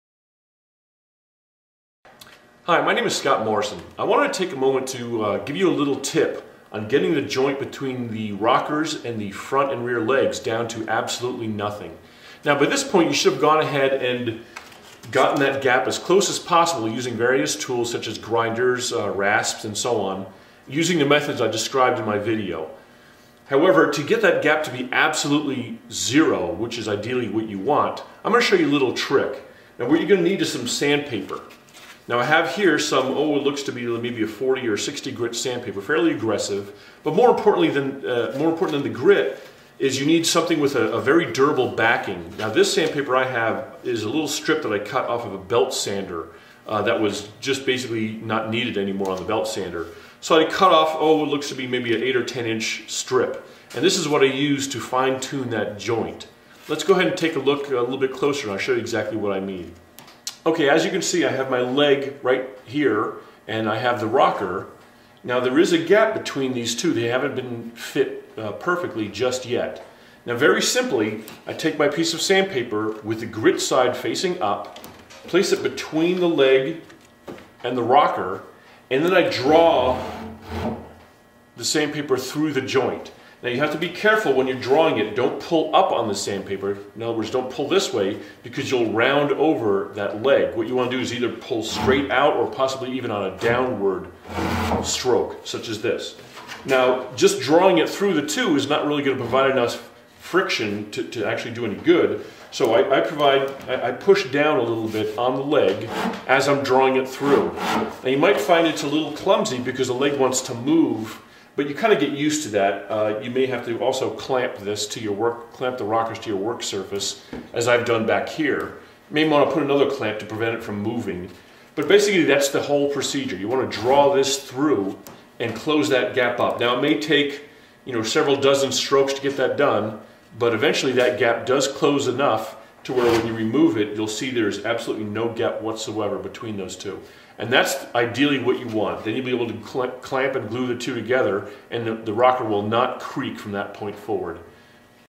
The "sandpaper trick"
This cool trick is useful when you need to join two dissimilar surfaces into a nice, perfectly matched joint.  I use it when attaching my rockers to their respective legs. It marries those surfaces joint to where it is virtually undetectible.  Thios method can be used in any similar situation where perfectly mathcing those surface give you a headache.